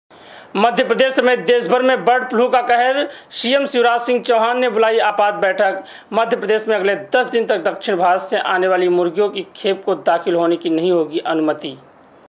MP: मध्य प्रदेश समेत देशभर में बर्ड फ्लू का कहर । शिवराज सिंह चौहान ने बुलाई आपात बैठक
मध्य प्रदेश समेत देशभर में बर्ड फ्लू का कहर,  शिवराज सिंह चौहान ने बुलाई आपात बैठक, मध्य प्रदेश में अगले दस दिन  तक दक्षिण भारत से आनेवाली मुर्गियों की खेप को दाखिल होने की नहीं होगी अनुमति